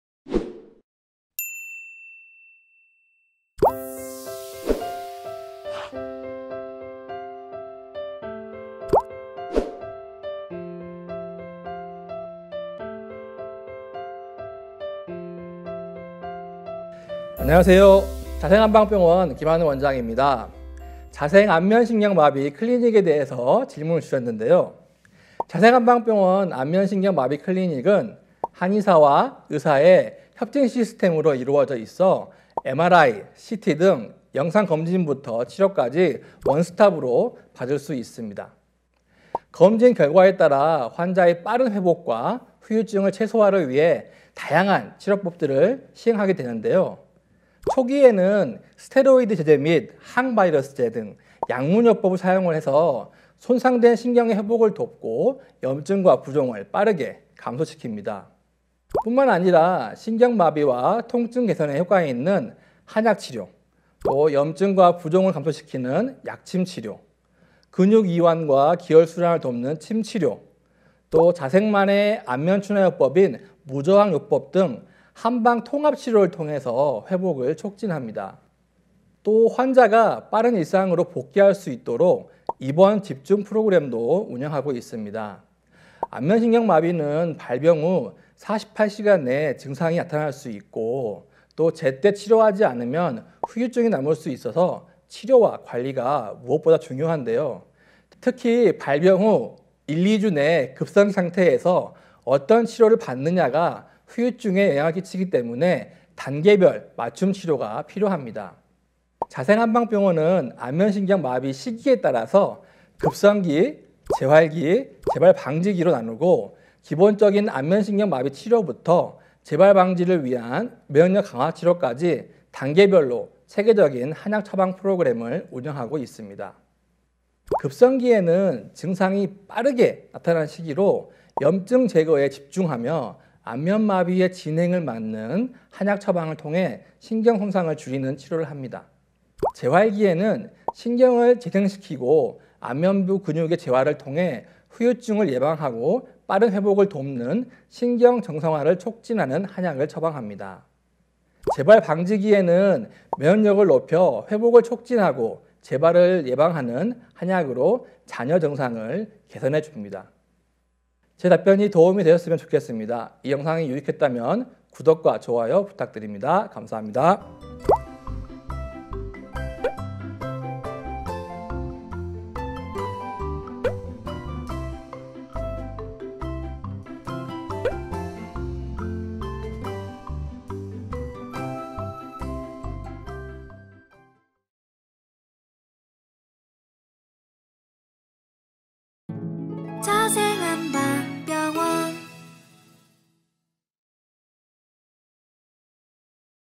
자생안면신경마비 클리닉 한큐에 알아보기 (안면마비 치료법 총정리)
명의에게 묻다 109편

안녕하세요! 여러분의 한방요정 자생한방병원입니다.
오늘은 자생한방병원의 김하늘원장님님께서
안면마비 한방 치료법에 대해서 자세히 설명 해주실거에요 ~!

안면마비는 초기,중기,말기에 맞춰 적절한 치료가 이루어져야
부작용을 최소화 할 수 있는데요!
각 시기 마다 어떤 치료법을 쓰고있고 그 효과는 무엇인지 궁금하시다면
영상 끝까지 시청해주세요 ~!

자생한방병원 안면신경마비클리닉은 한의사와 의사의 협진 시스템으로
이뤄져 있어 MRI, CT, 등 영상검진부터 치료까지 ONE-STOP으로 받을 수 있습니다.